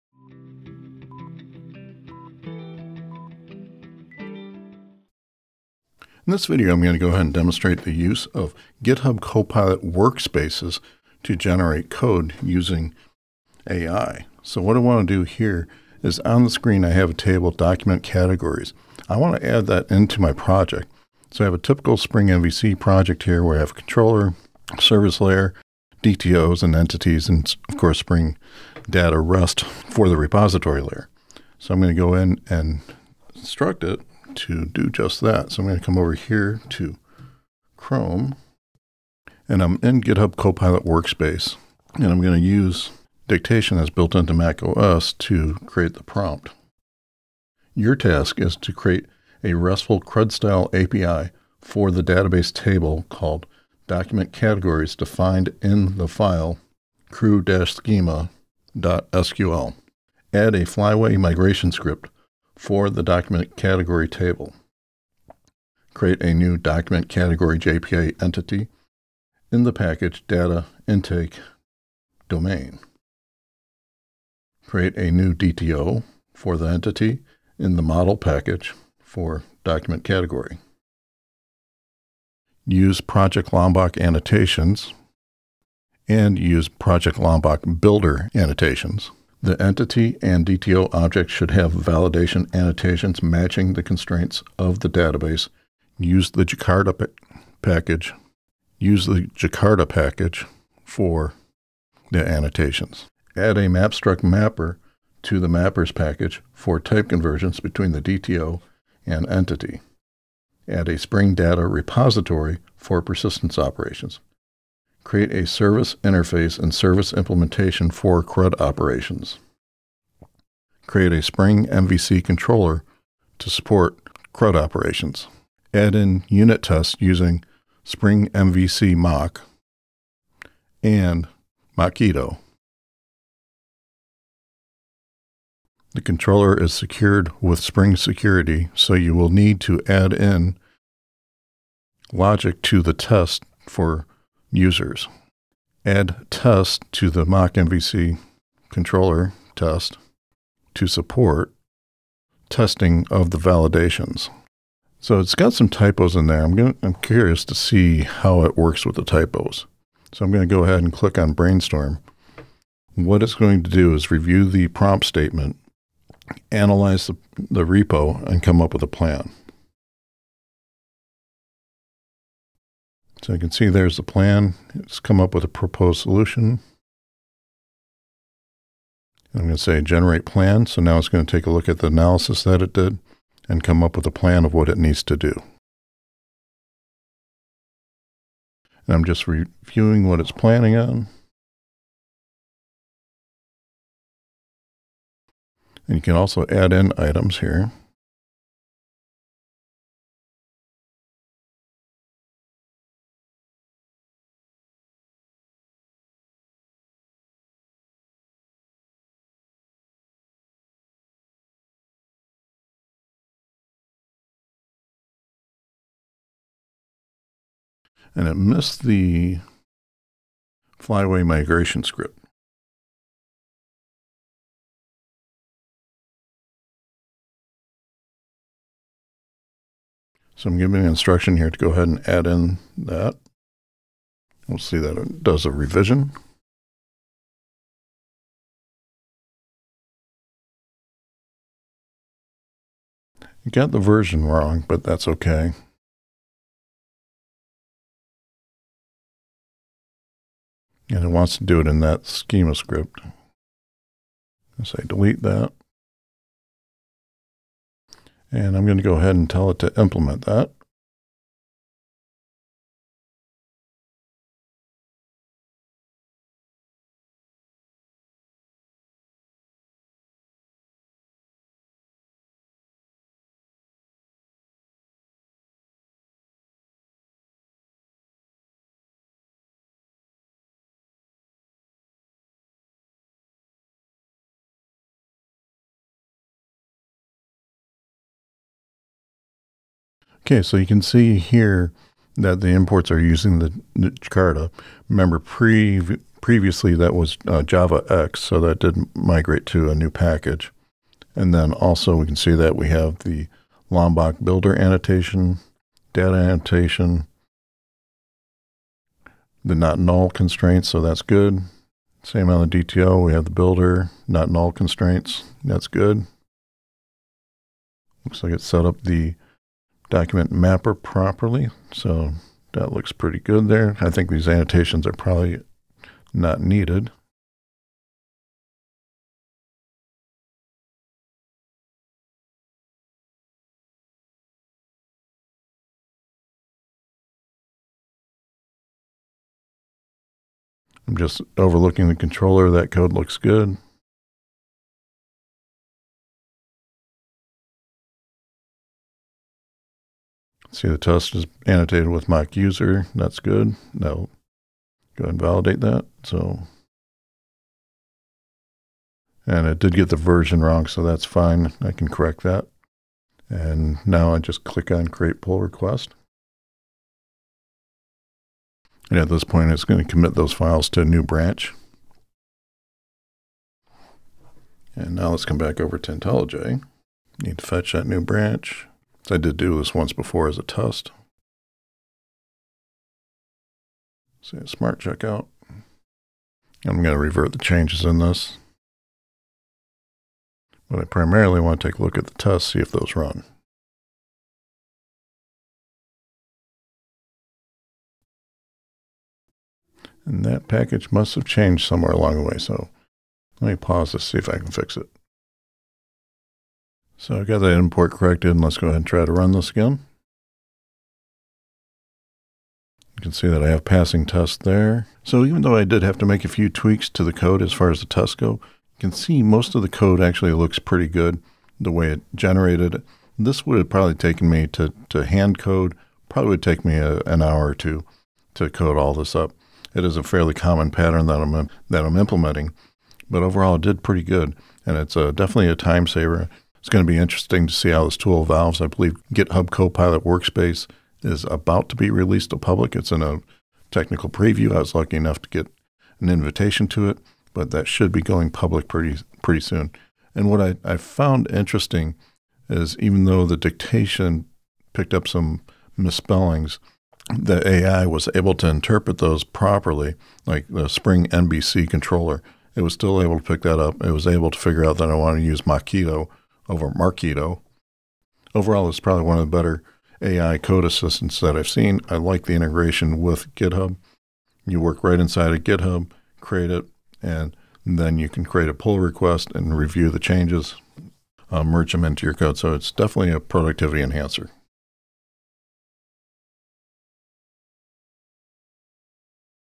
Supercharge Spring Boot Development with GitHub Copilot Workspace
Are you ready to take your Spring Boot development to the next level? 🚀 In this video, I show you how to supercharge your coding workflow using GitHub Copilot Workspace, the AI-powered assistant that writes code for you!

What if you could just talk and have an intelligent assistant write code for you? But there's more. You can direct your assistant to make changes and adjustments to their work.  And when ready commit those changes and create a pull request for your review.

In this video, I direct Copilot to an SQL create table statement in a file in my repository and then ask it to create a Spring MVC RESTful API. We don't create just a controller. We perform a complete implementation. Copilot writes the JPA Entity, the DTO POJO, a MapStruct Mapper, a Spring Data JPA repository, a service and implementation for persistence operations, AND Spring MockMVC unit tests with Mockito.

In just 10 minutes I write a complete RESTful API, a task that used to take HOURS!

Ready to Learn Spring Boot?